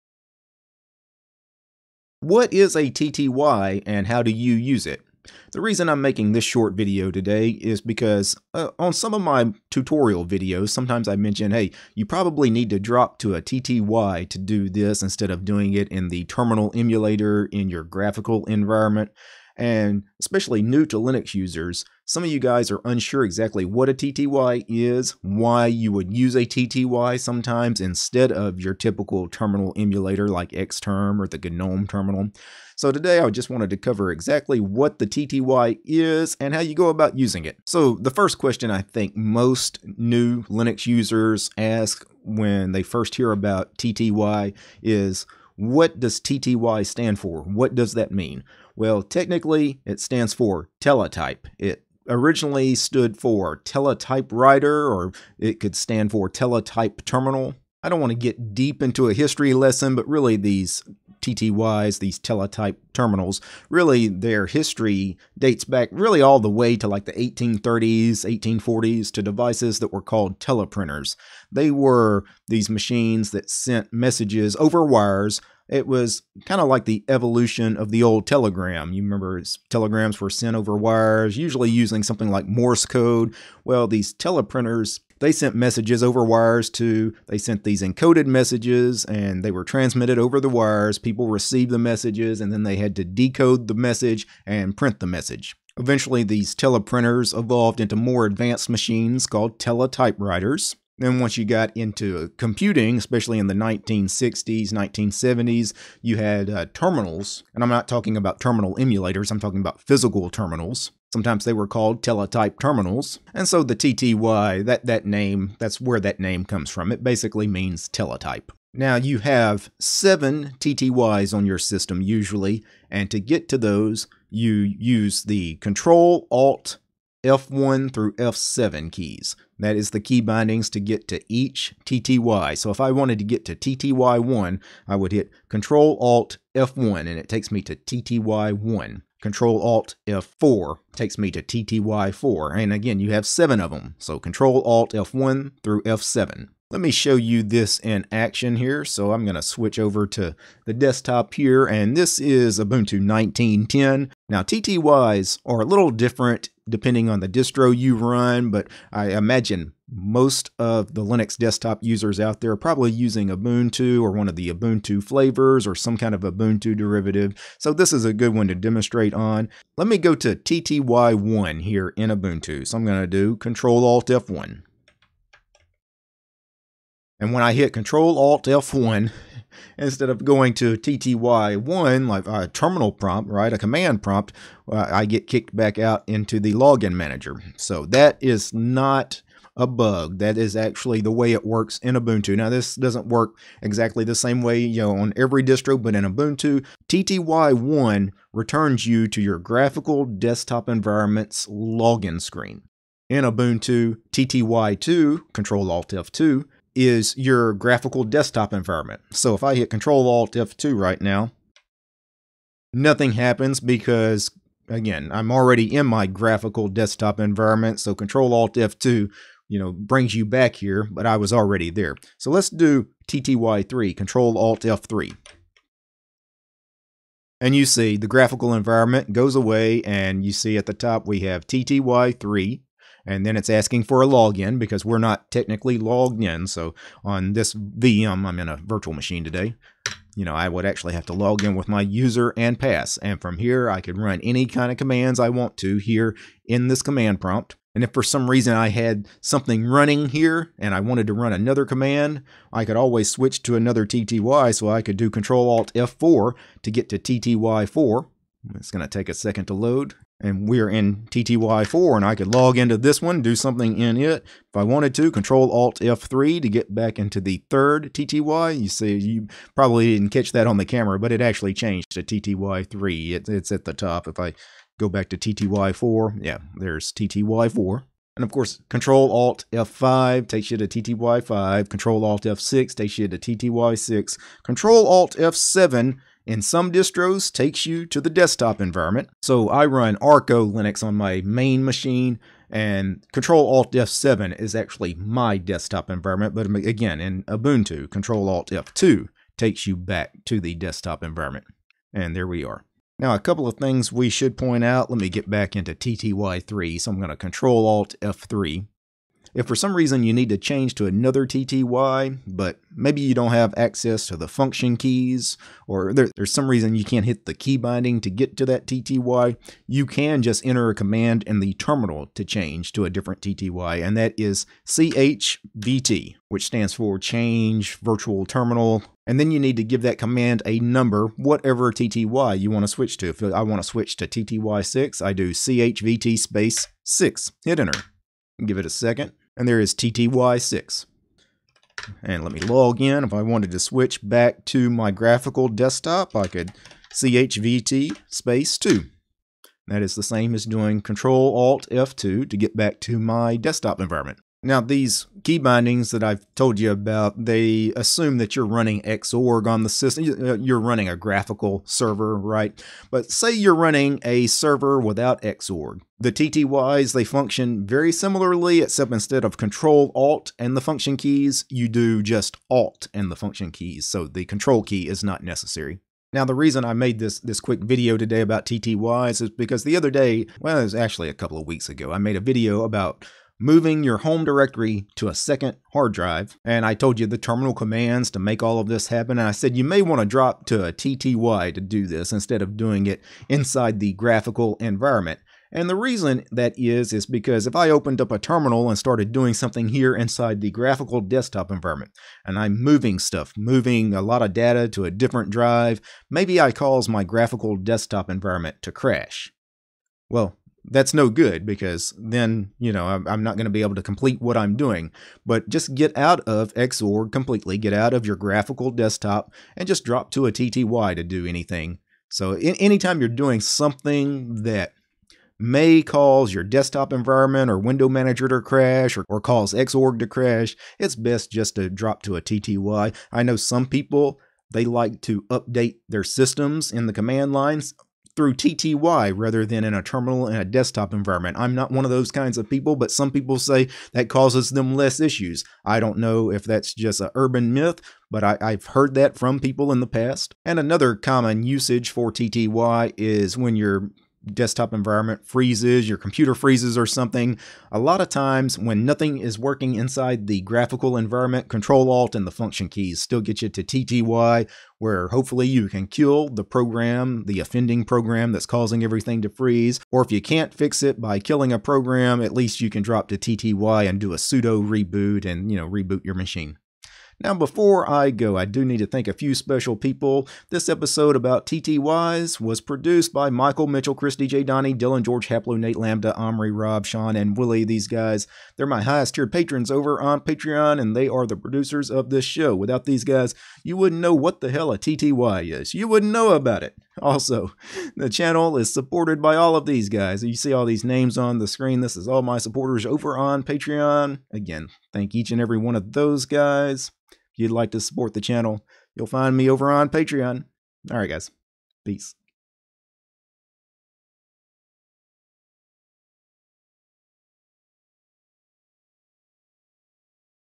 What Is A TTY And How To Use It
What is a tty and how do I use it?  I've been asked this question a few times in recent weeks, so I thought I would make a brief video discussing this.